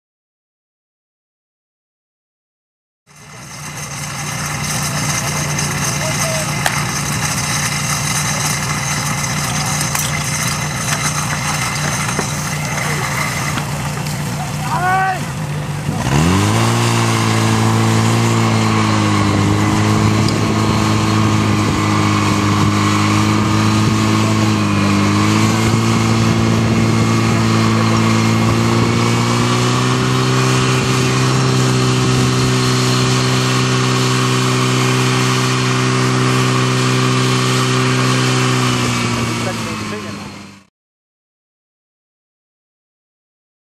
SDH Rožnov na Zlaté proudnici v Jezbinách 2015
Útok družstva mužů SDH Rožnov na Zlaté proudnici v Jezbinách konané dne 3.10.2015. Umístění: 3. místo.